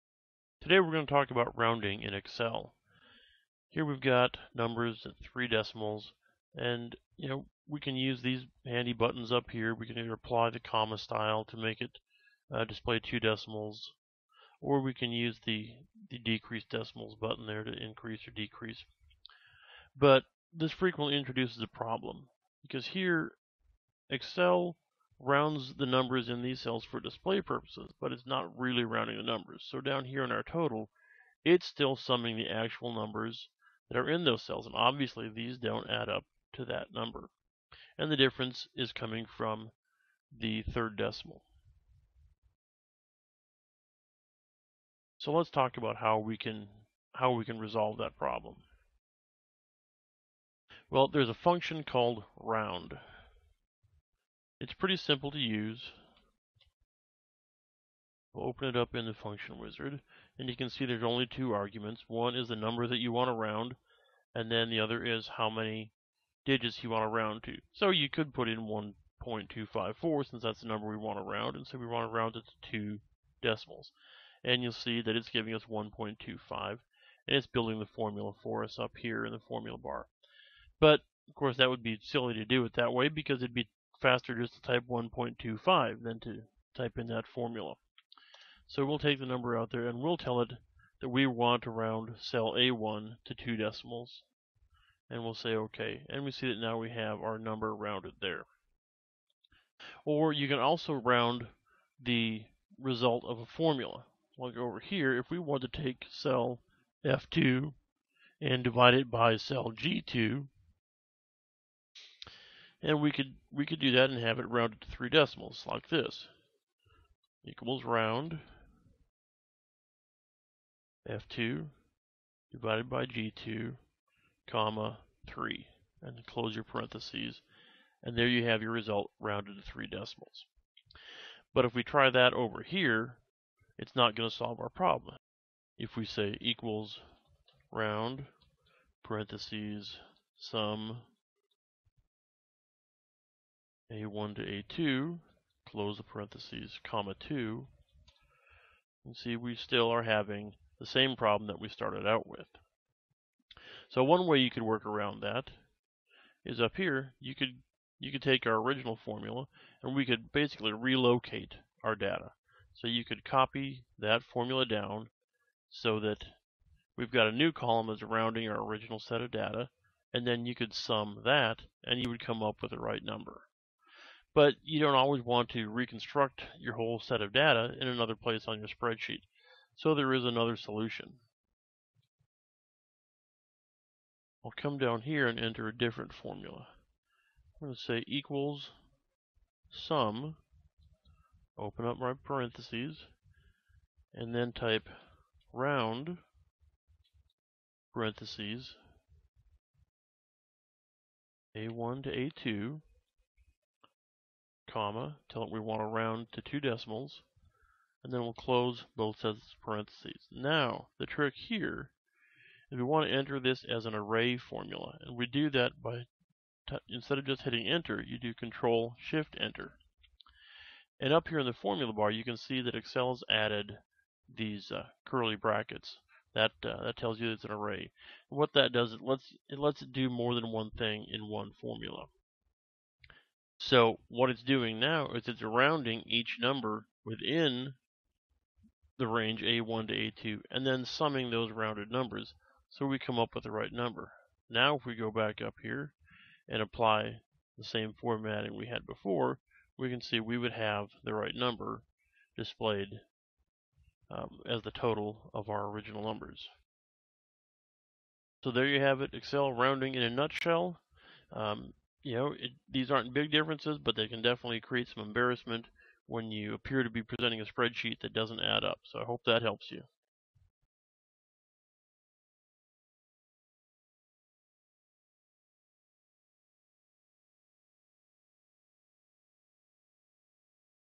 Rounding Numbers in Excel
It's important to understand the difference between Excel number formats that limit the number of decimals displayed, and actually rounding numbers. It some situations the difference is the result can be critical.